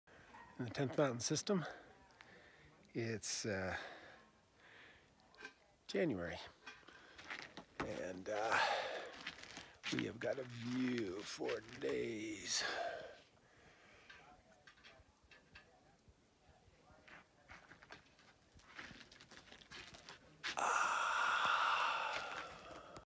2019 Benedict 100: Margy's Hut
The 2019 Benedict 100 took place on Jan 27 - Feb 1, 2019. It is a 6-day backcountry ski trip that goes from Aspen to Vail, Colorado, and visits 5 backcountry huts. This video was filmed on the deck of Margy's Hut, named after Margaret McNamara, wife of Robert McNamara.

"Built in 1982, Margy's Hut (along with the McNamara Hut) was one of the first two huts in the 10th Mountain system. Hut construction was funded by Robert McNamara (former secretary of defense) and friends, in memorial to Robert's wife Margy. Margy's Hut is located on a lightly treed hillside just below timberline at 11,300', about 10 miles northeast of the city of Aspen, Colorado. The hut has beautiful views of the Elk Mountains to the south.”